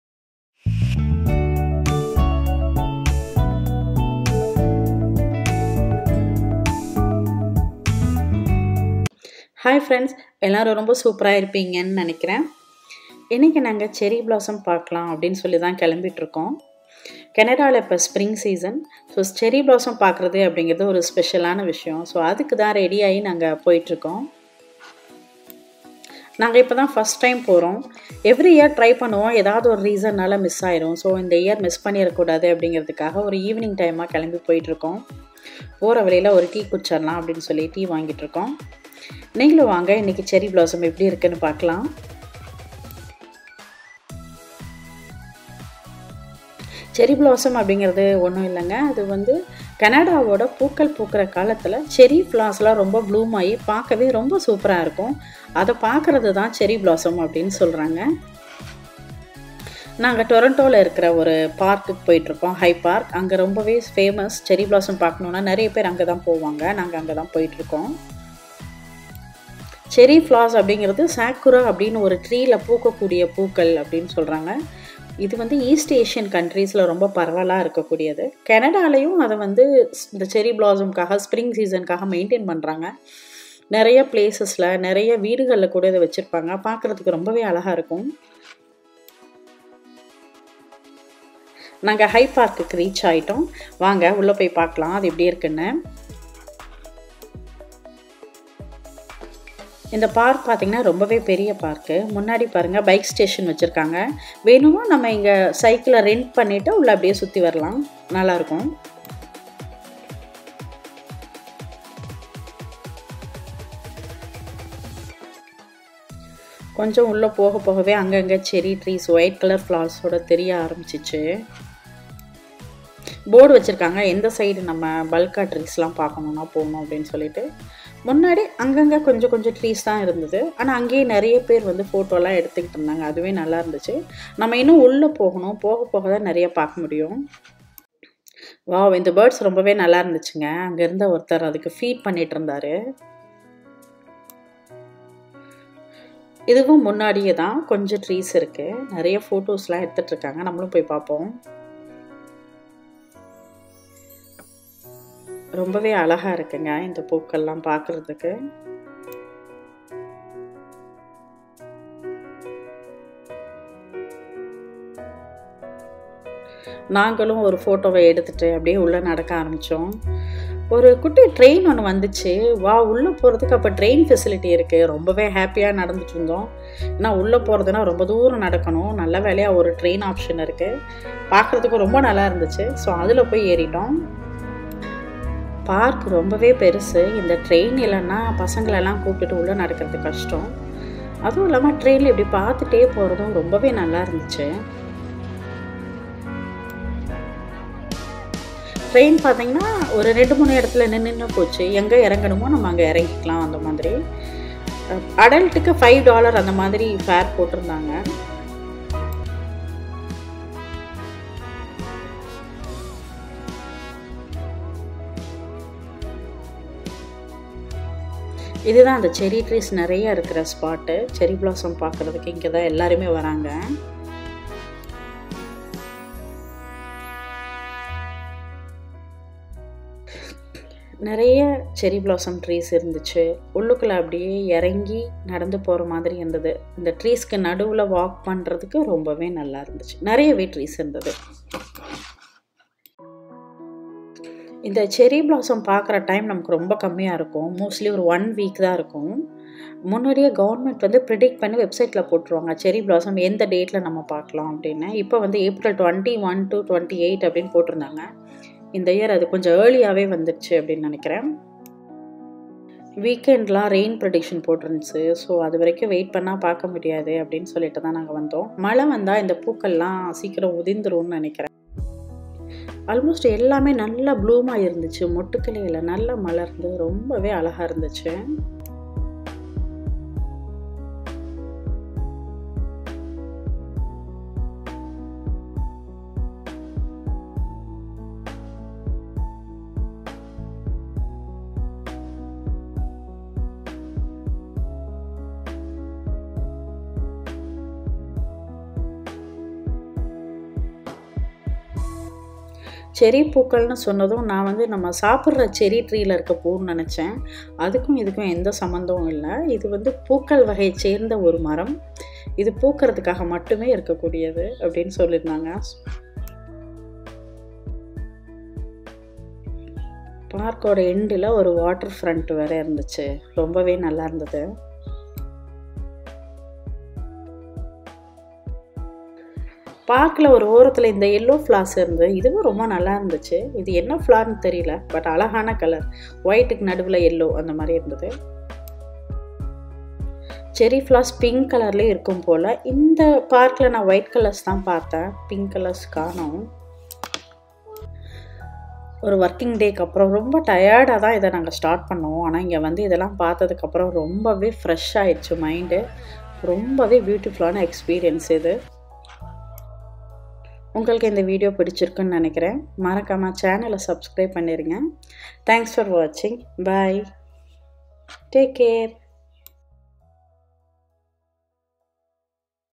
Cherry blossom in canada | High park toronto | beautiful ride family time
Quick car road trip to see cherry blossom in Canada 2023 💕🍁🇨🇦🌸🌺
High park in Toronto canada downtown views
Train ride in park
Bike riders
Lovely blooming White flowers
Famil spending relaxed evening with nature
Spring season in Canada
Thomas kids train 🚂
Photoshoot
Evening sunset with white sakura flowers

Canada Tamil vlog 2023